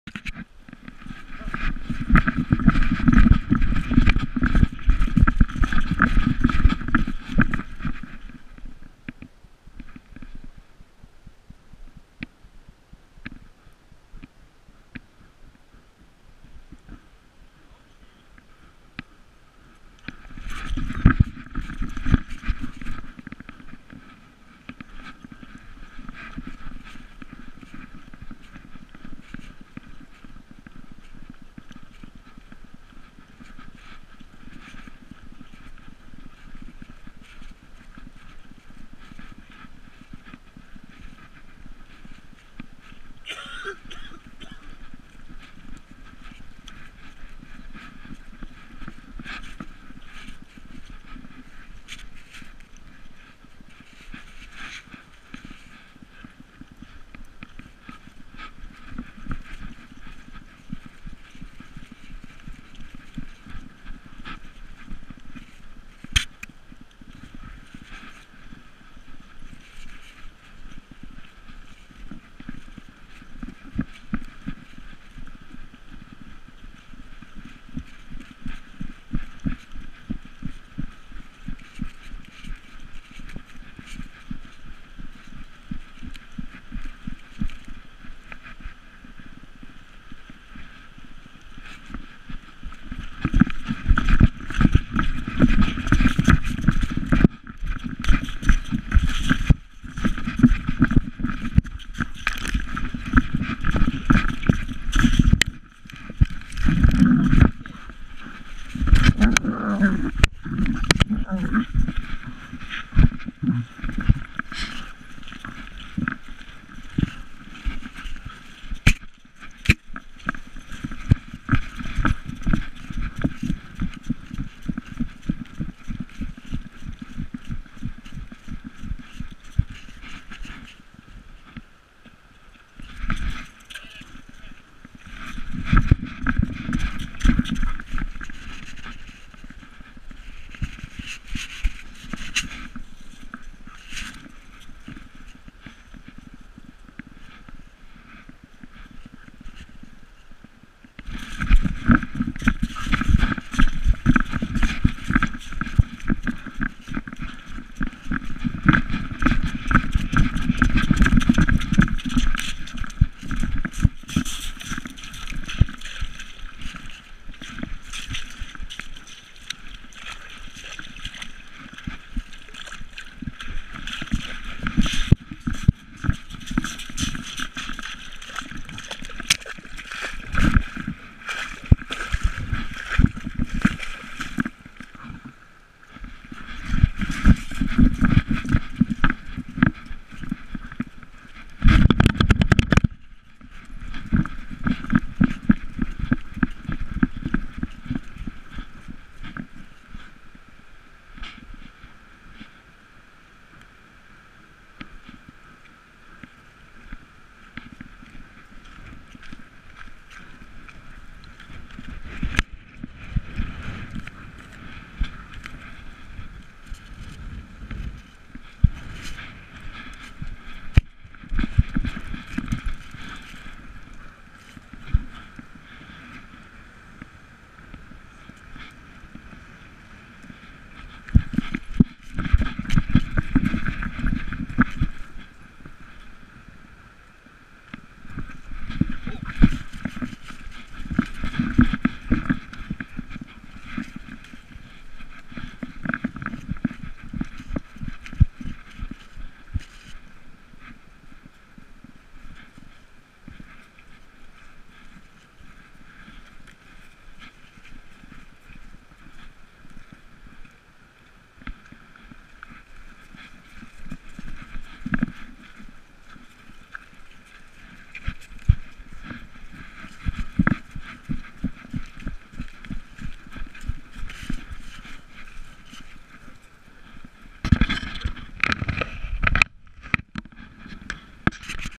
Through the eyes of a wolfhound
Gopro attached to my wolfhounds collar.